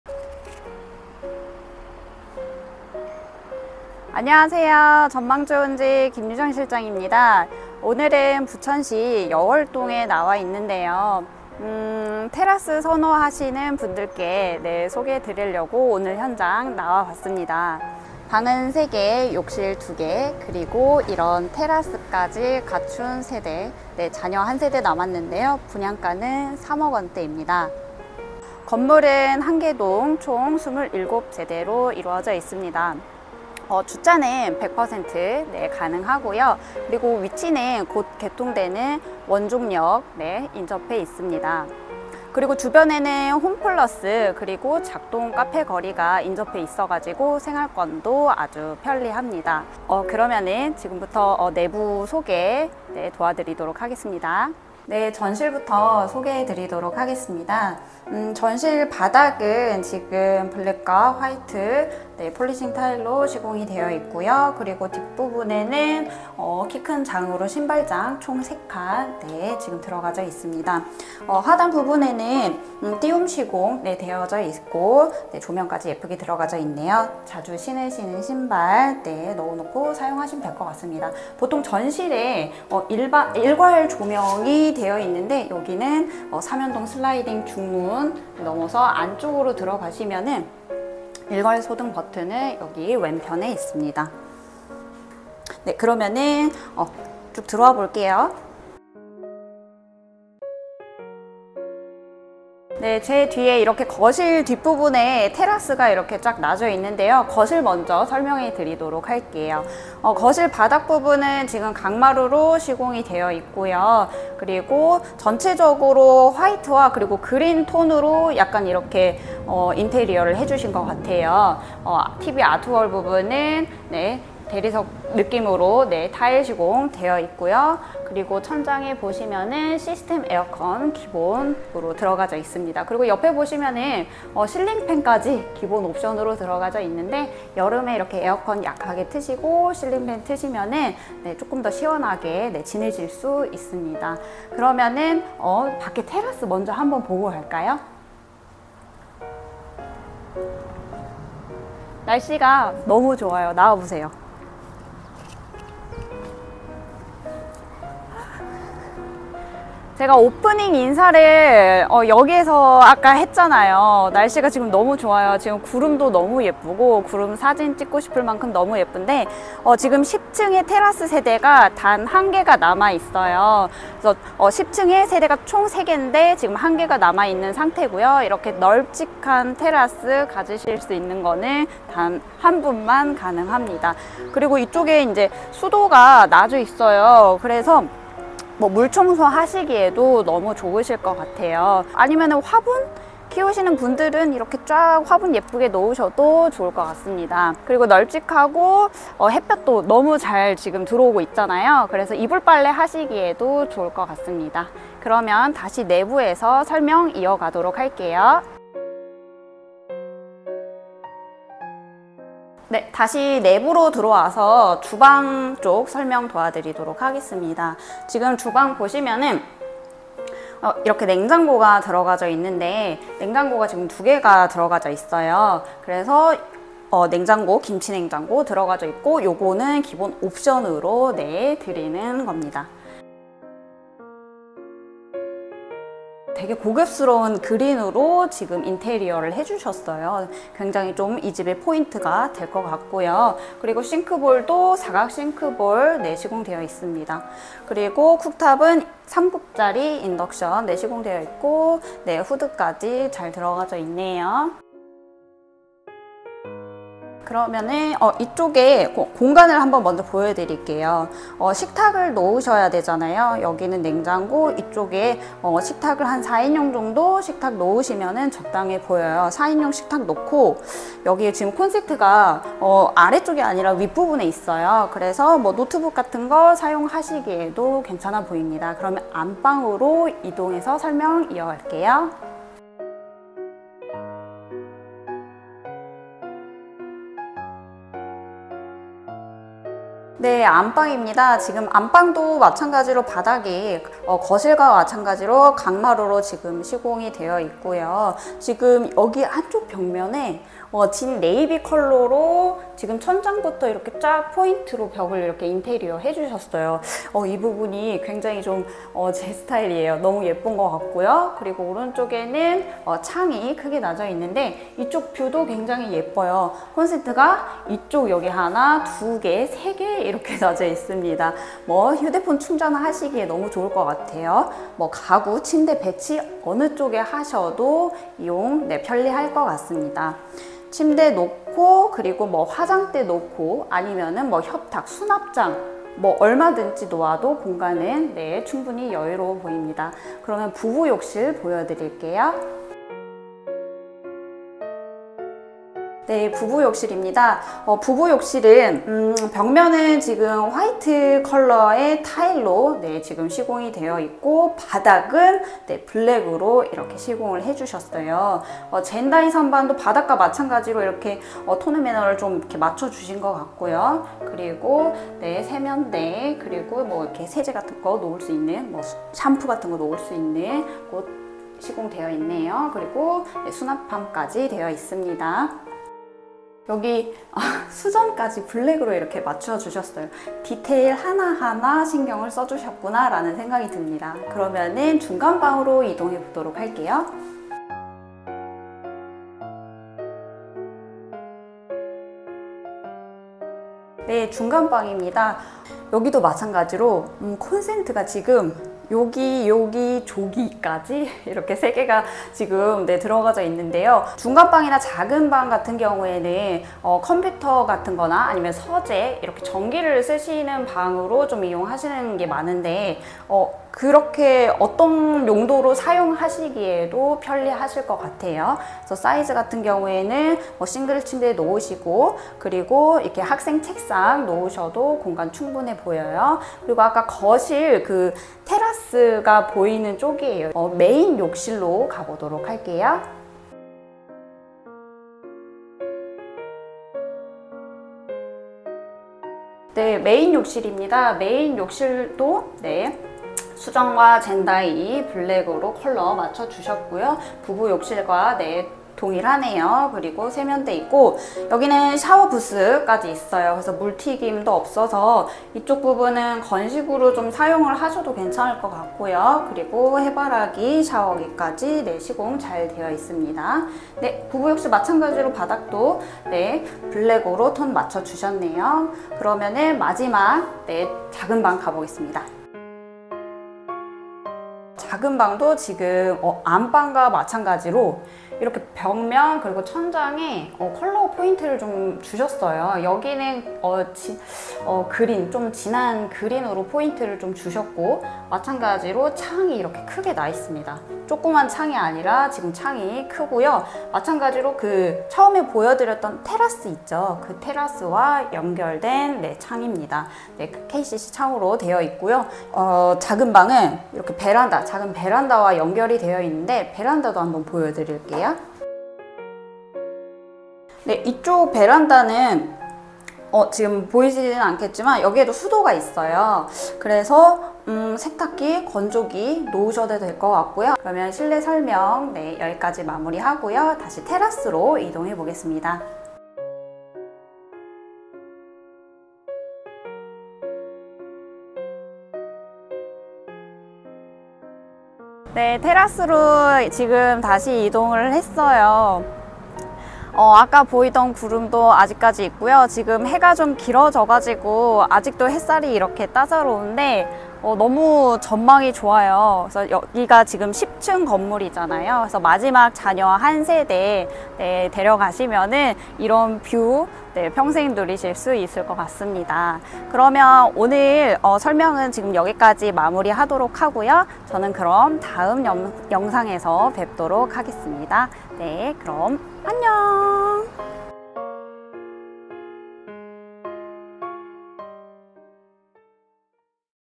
[부천테라스아파트] (No.755) 3억대로 야외테라스까지 드립니다. 부천시 여월동 3룸 아파트⭐잔여1세대 [여월동신축아파트][부천신축빌라]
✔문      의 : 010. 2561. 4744

✔위      치 : 부천시 여월동
✔구      조 : 20평형 / 방3개 화장실2개 베란다1개 통테라스 1개
✔특      징 :
- 3억원대 20평형 통테라스 + 잔여 딱 1세대!
- 통테라스에 수도가 시공되어있어 물청소 가능
- 거실 + 중간방 + 작은방이 통테라스와 창으로 연결되어있을 정도로 긴테라스
- 막힘없는 시원한 전망
- 원종역 7호선 부천종합운동장역 더블역세권 여월동 신축아파트
- 주차 100% 가능

✔옵      션 : 아일랜드식탁, 빌트인 시스템에어컨 2대, 삼성 비스포크 냉장고, 김치냉장고
✔편의시설 : 원종역 도보 10분, 초중고 도보통학 가능, 홈플러스 경찰서 베르네천근린공원 작동카페거리 인접

00:00 현장소개
00:55 내부소개
01:42 거실
02:30 테라스
03:33 주방
04:49 안방
05:45 부부욕실
06:33 중간방
07:19 메인욕실
07:54 작은